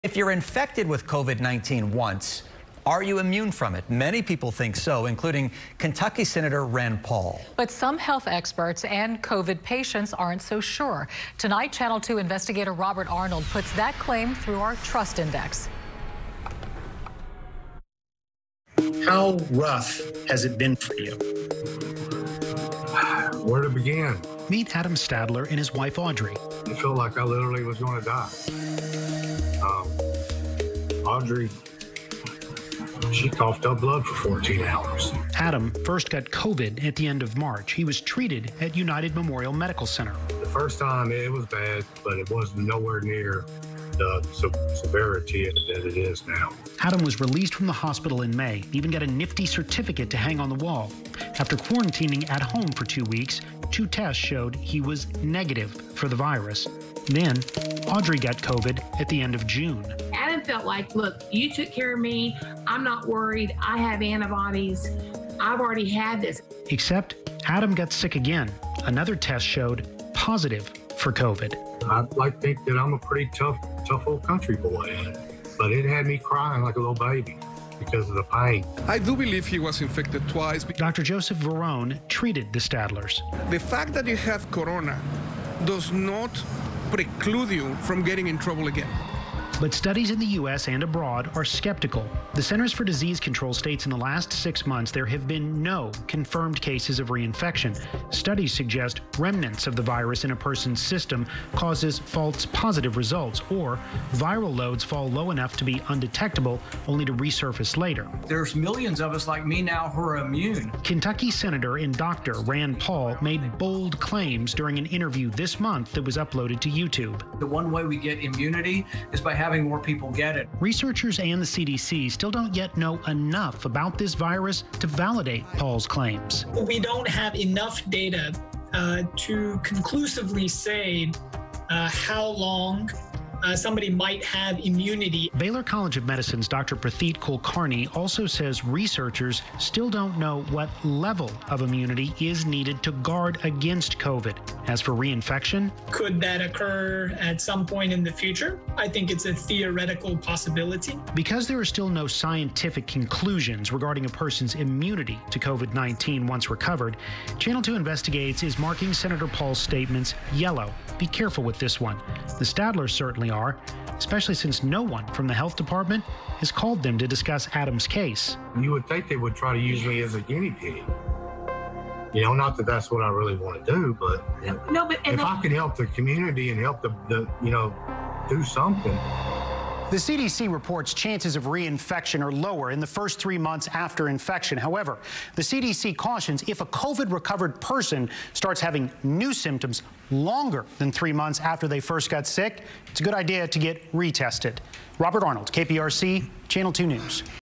TRUST INDEX: Can you get COVID-19 twice?
Can you get COVID-19 twice? Are you immune from getting it once you are recovered? These are questions we’ve received have many times from viewers. A prominent Senator recently reiterated claims about immunity we put those claims to our trust index.